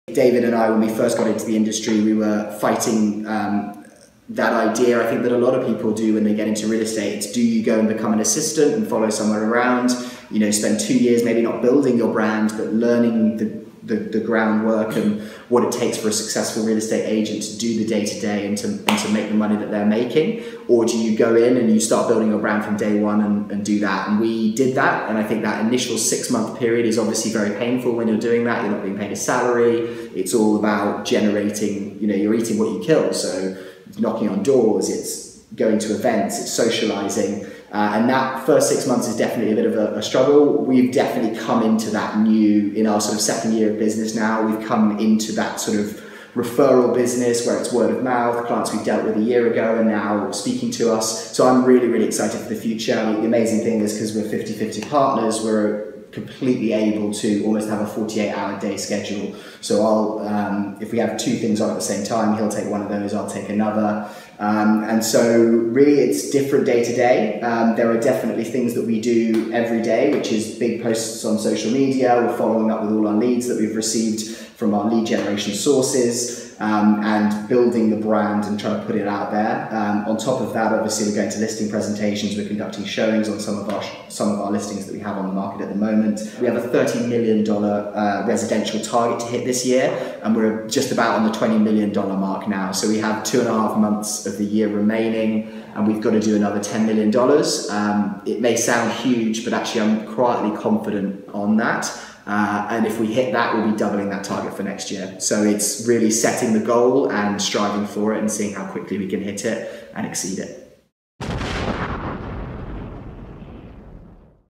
The Day-to-Day Grind of a Real Estate Agent | Edward Hutchinson Tip 14
LA Realtor, Edward Hutchinson, shares how he organizes his day-to-day schedule with real estate partner, David Johnstone.